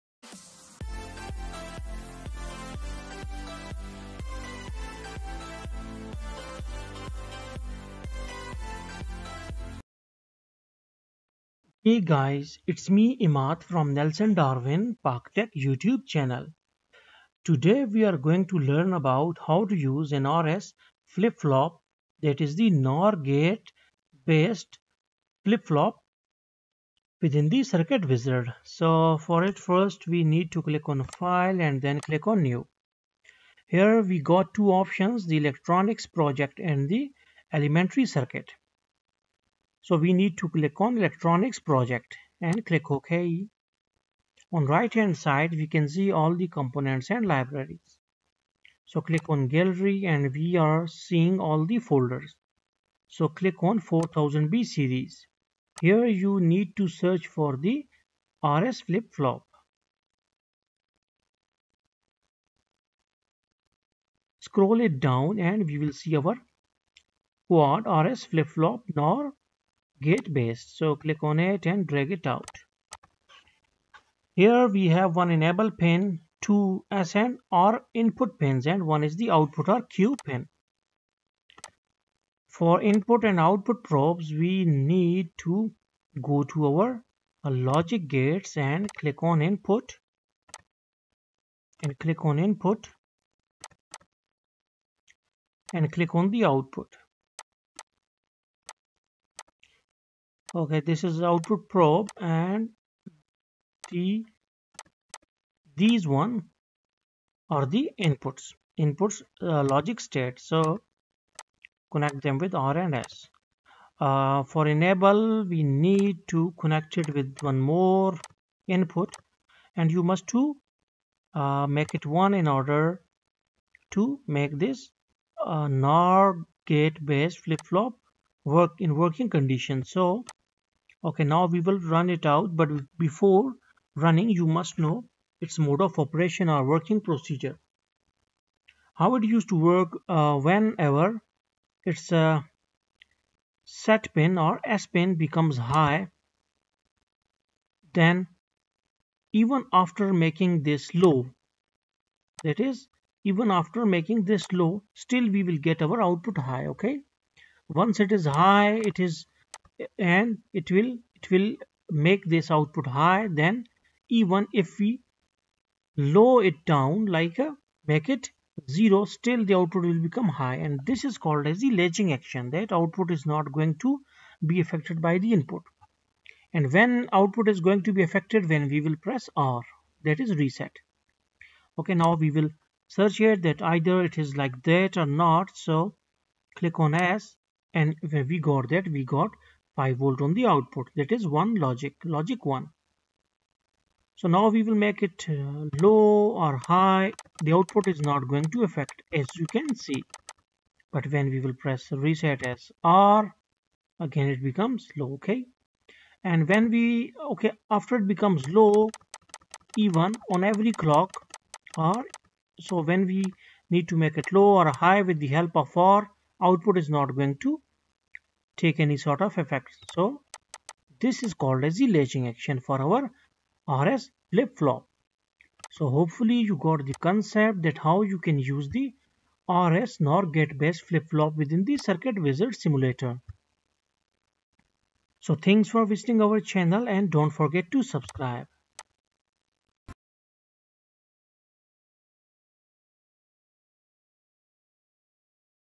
RS Flip Flop in circuit wizard | Simulation of RS Flip Flop in circuit wizard
1. RS Flip Flop in circuit wizard.
2. Simulation of RS Flip Flop in circuit wizard.
3. tutorial on how to use RS Flip Flop in circuit wizard.